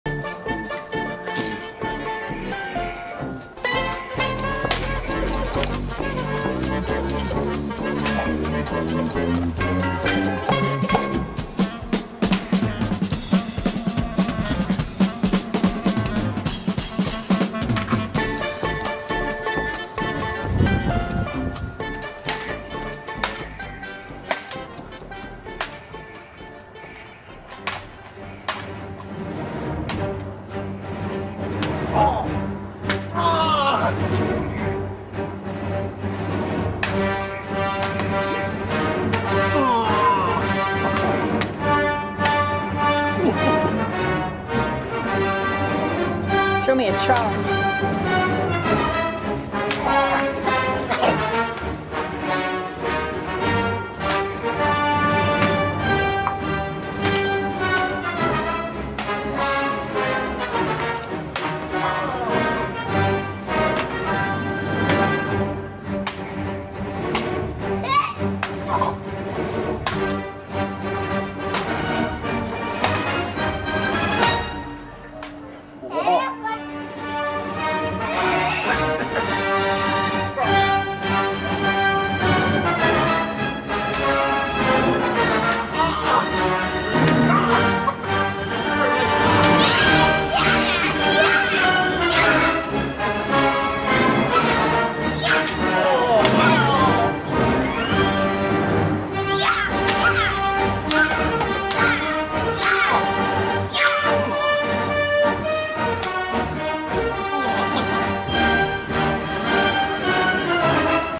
Zach is 3-Year-Old Darth Maul (Lightsaber Duel)
He's ready in October if Disney tries anything funny like swapping Vader out for Maul.